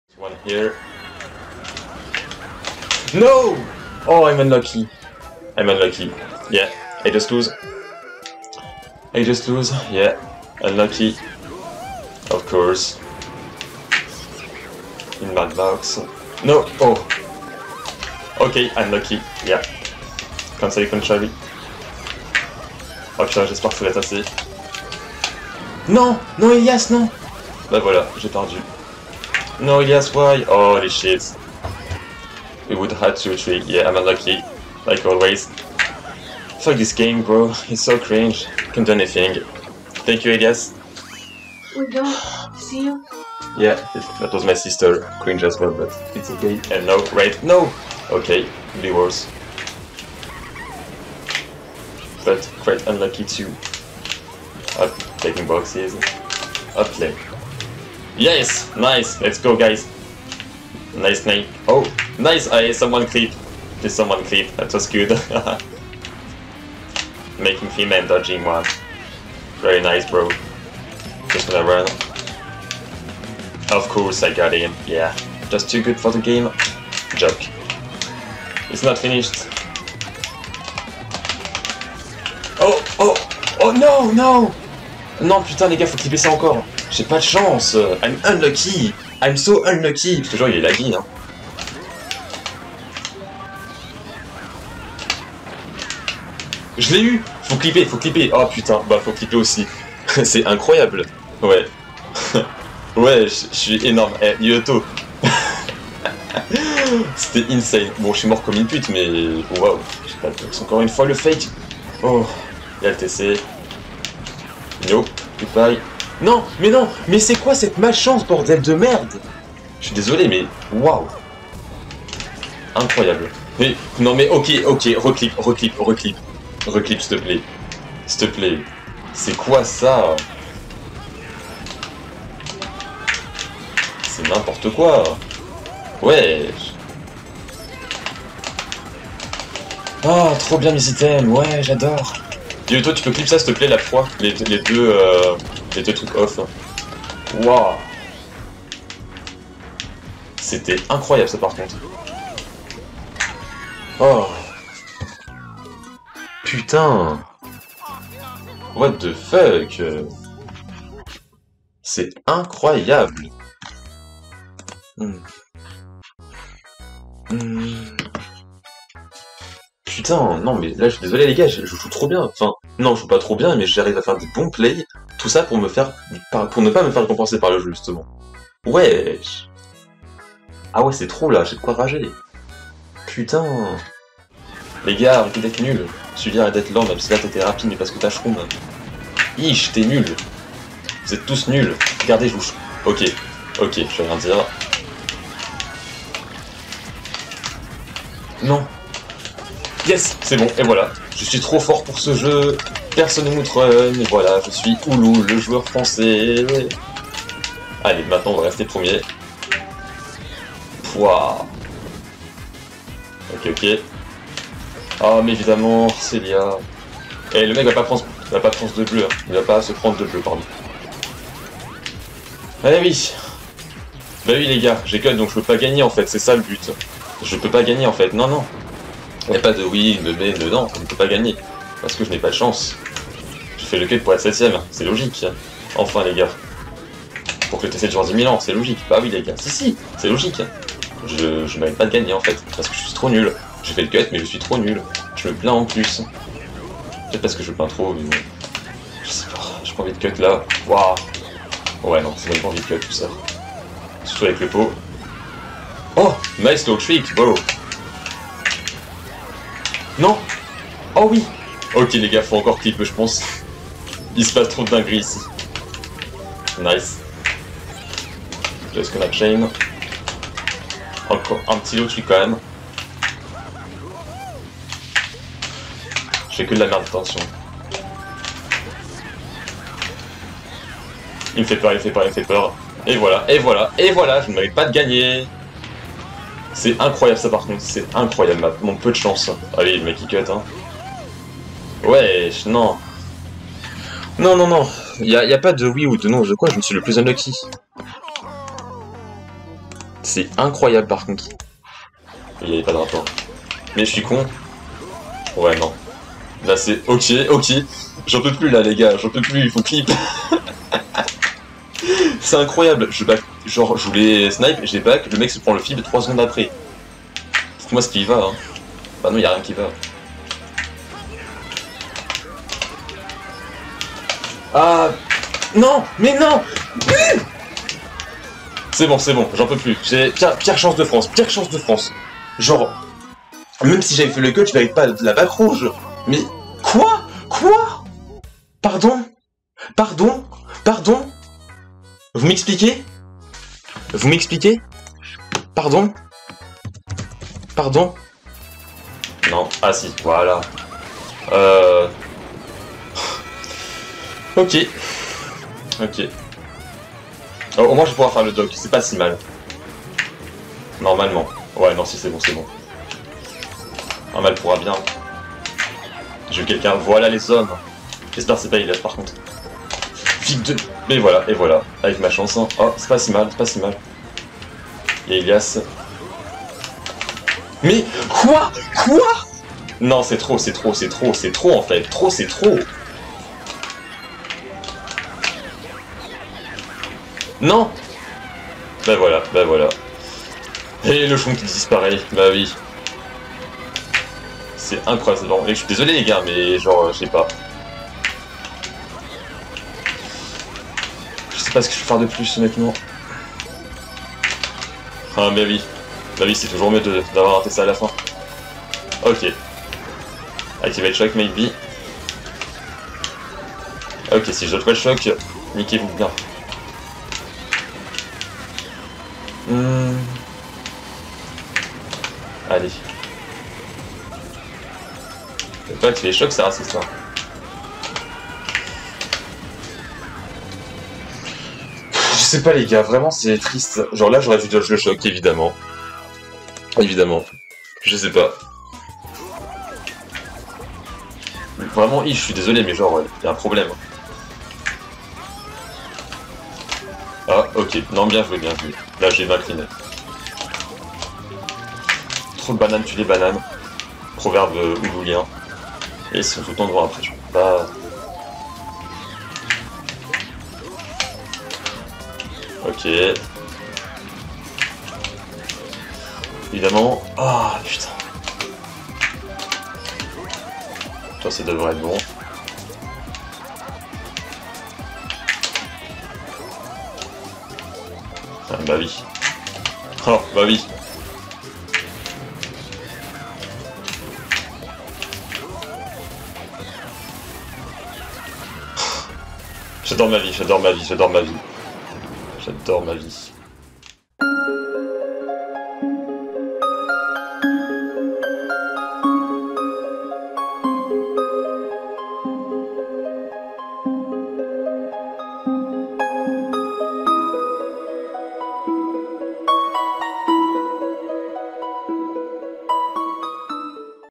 Ouloul le rageux (Best Of d'un mogi abberrant)
Voilà je devais poster ça pcq la bon c'est insane
profitez bien de ces doss !
avec mon mate on fait 5ème à la fin mais whoa pire mogi de ma vie quoi

Bon visionnage !